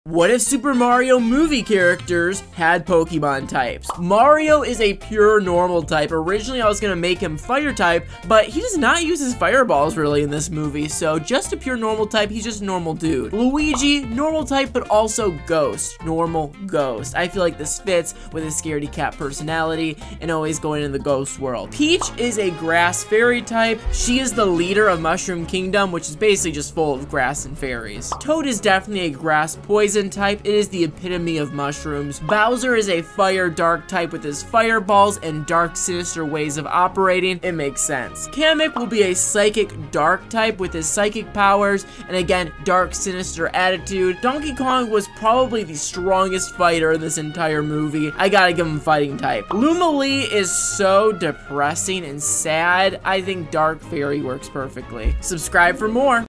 What if The Super Mario Bros. Movie Characters had Pokemon Types
Today we discuss what pokemon types characters in the Super Mario Bros. Movie would have:  Mario, Princess Peach, Luigi, Bowser, Kamek, Toad, Donkey Kong, Luigi, Lumalee.